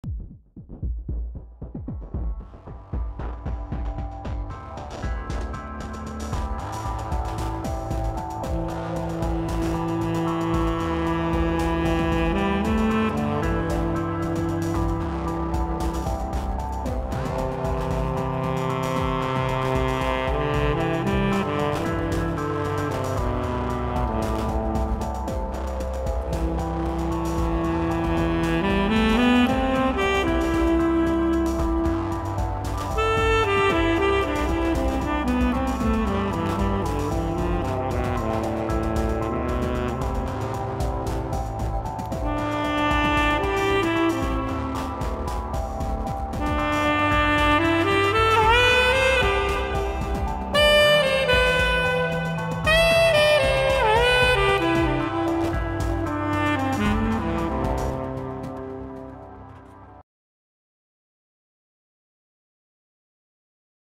Saxophone and Synthesizers
A quick improv session while trying out my new Qu-bit Bloom!